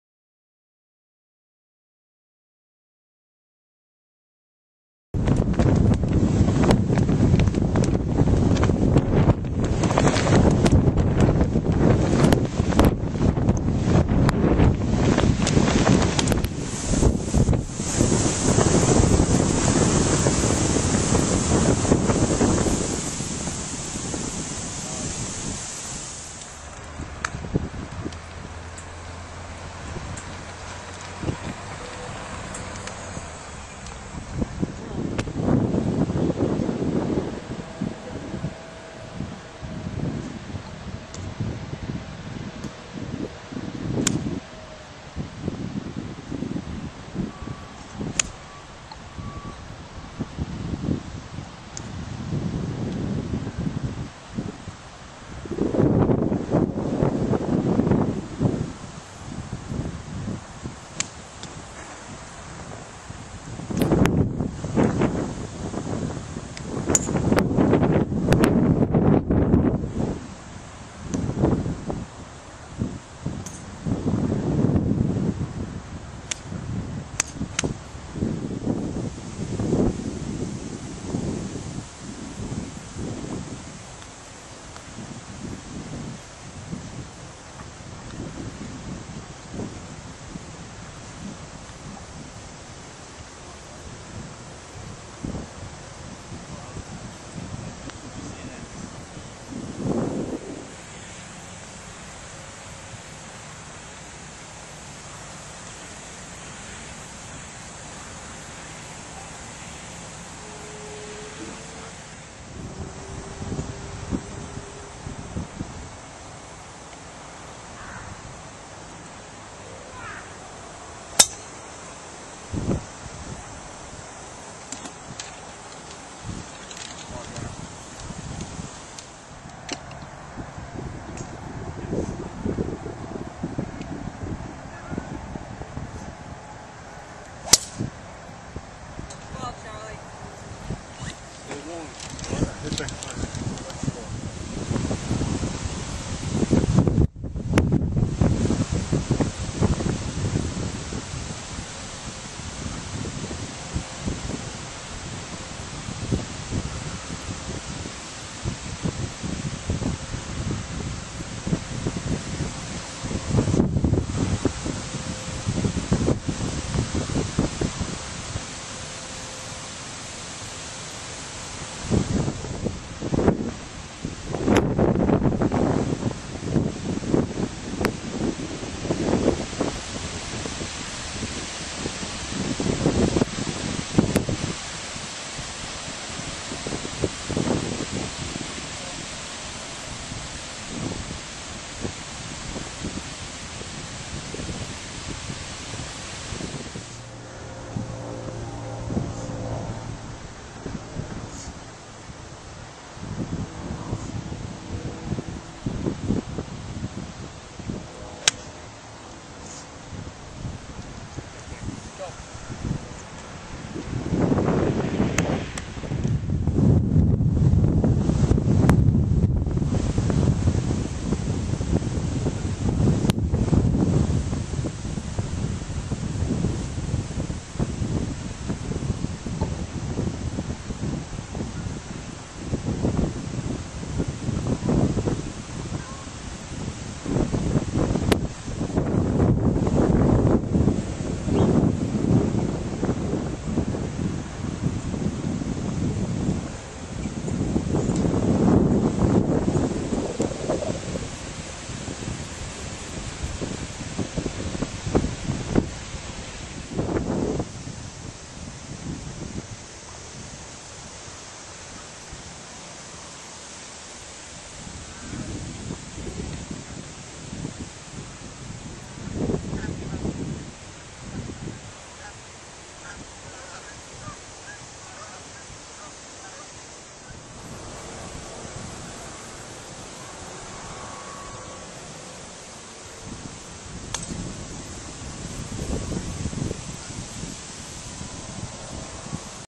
John Dallio Memorial Highlights - Part 1
Highlights from the third round of the John Dallio Memorial at Ruffled Feathers GC on Sept. 28, 2009. Greg Doherty was medalist and the Blue Demons won the team title.